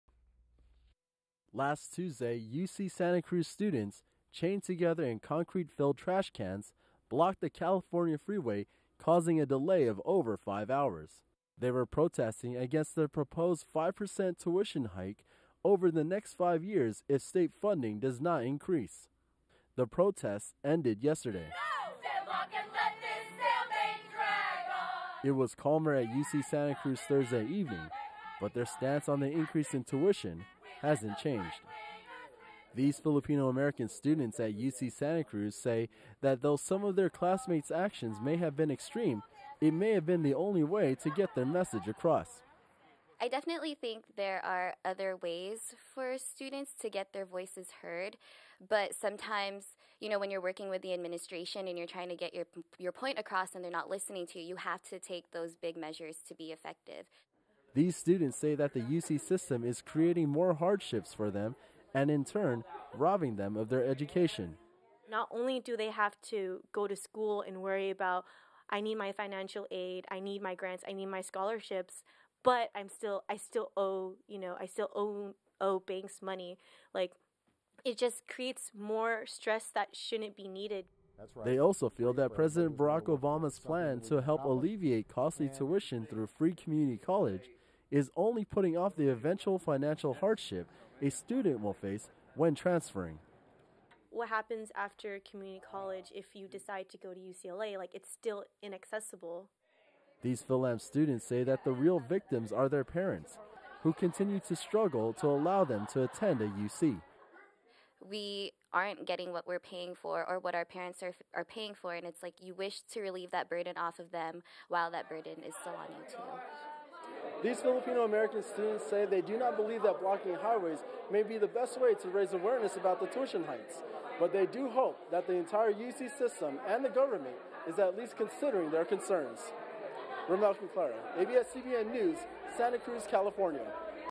Fil-Am UC students say tuition hikes rob them of education
Filipino American students at UC Santa Cruz say that though some of their classmates actions may have been extreme but it may the only way to get their message across.